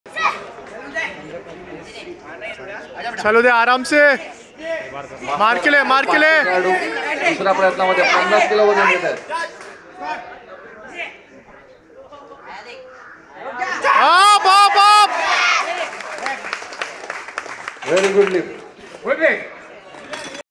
Uday squats 55kg at 30kgs BW 11 yrs old
Competition: Mumbai District
Name: Uday Singh
Age: 11 years
Lift: Squat of 55kgs
Bodyweight: 30Kgs
Club: Mega Creation